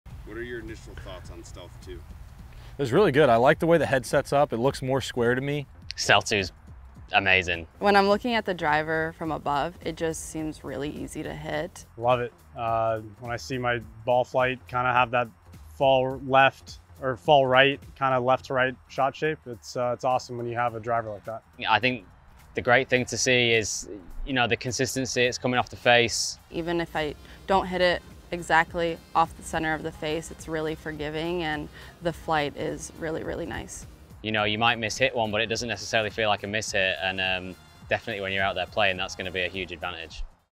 Team TaylorMade's First Impressions of Stealth 2 Driver | TaylorMade Golf
Scottie Scheffler, Collin Morikawa, Tommy Fleetwood, and Nelly Korda take you through their first impressions of Stealth 2 Driver. They explain the look at address, the way the ball comes off the face, and how the miss-hits don't really feel like miss hits.